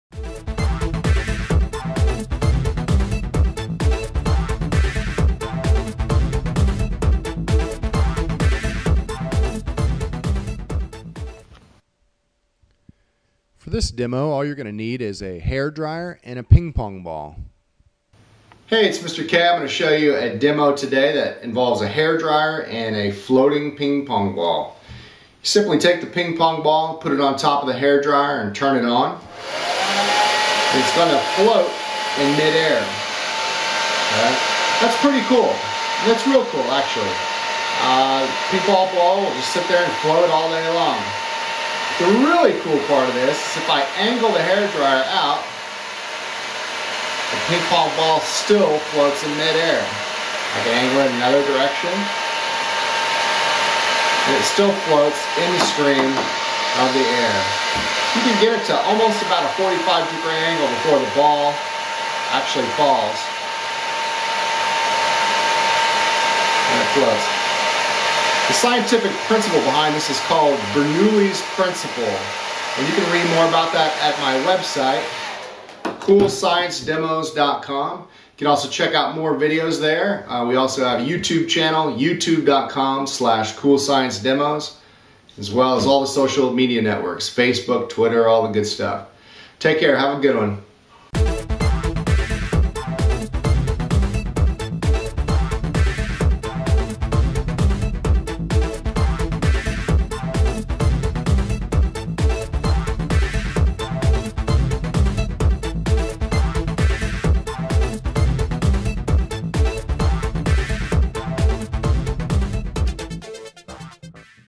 Awesome Floating Ping Pong Ball - Bernoulli's Principle
Make a ping pong ball float in mid-air with a hair dryer.  This science demo teaches Bernoulli's Principle.  For more cool science demos check my